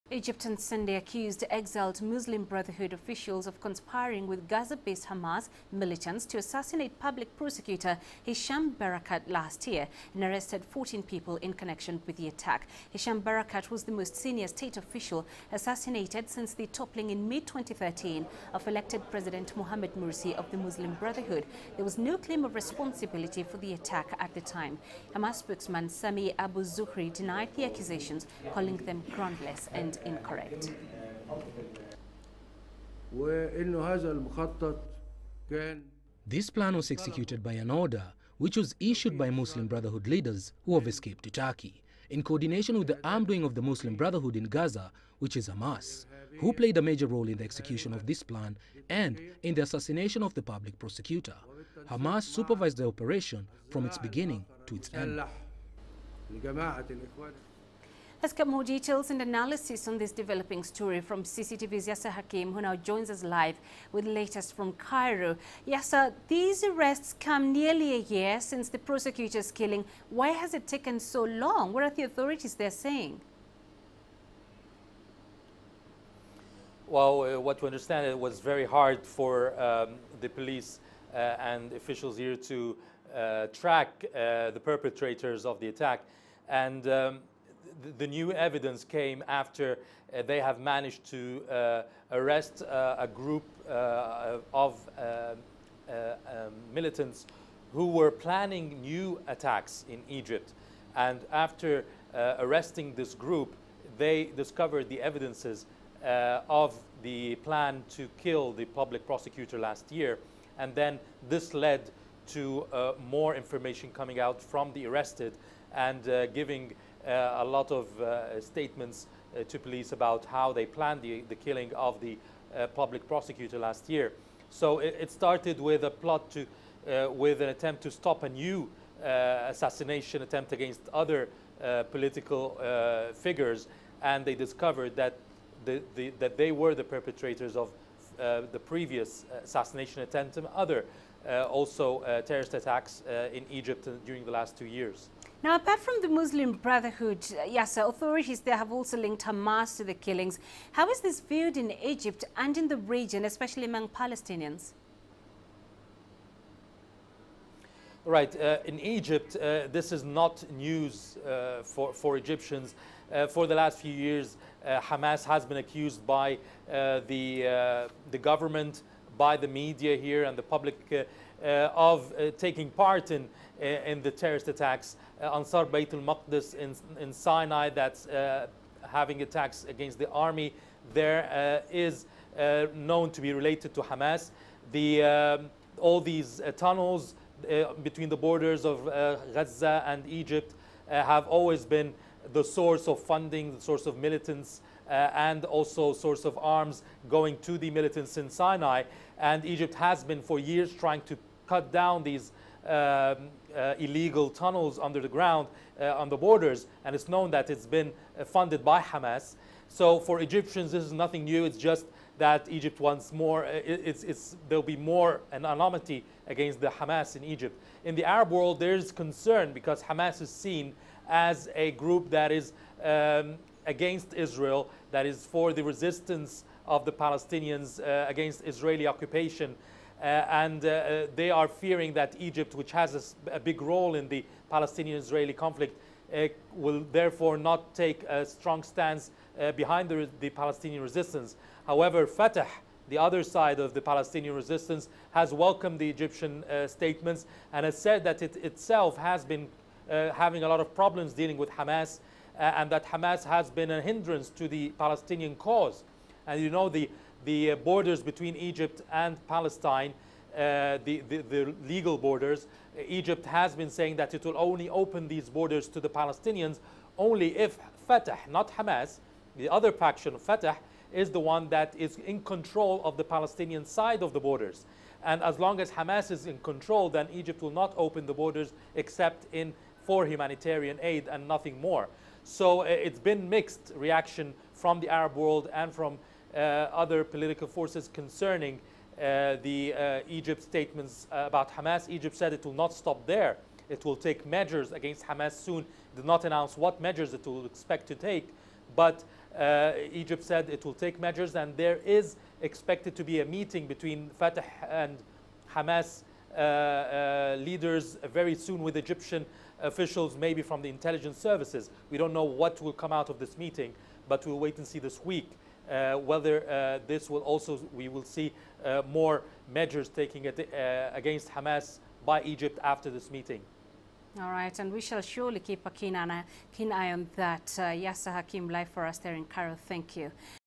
Egypt authorities accuse Muslim brotherhood of killing prosecutor
Egypt on Sunday accused exiled Muslim Brotherhood officials of conspiring with Gaza-based Hamas militants to assassinate Public Prosecutor Hisham Barakat last year and arrested 14 people in connection with the attack. Hisham Barakat was the most senior state official assassinated since the toppling in mid-2013 of elected president Mohamed Mursi of the Muslim Brotherhood. There was no claim of responsibility for the attack at the time. Hamas spokesman Sami Abu Zuhri denied the accusations, calling them groundless and incorrect.